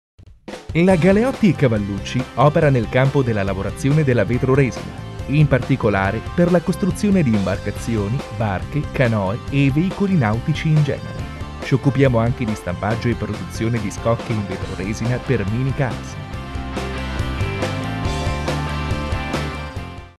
GALEOTTI & CAVALLUCCI FORLÌ (FORLÌ-CESENA)
GALEOTTI & CAVALLUCCI 2, V. KING Tel La GALEOTTI & CAVALLUCCI opera nel campo della lavorazione della vetroresina, in particolare per la costruzione di imbarcazioni, barche, canoe e veicoli nautici in genere.Ci occupiamo anche di stampaggio e produzione di scocche in vetroresina per minicars.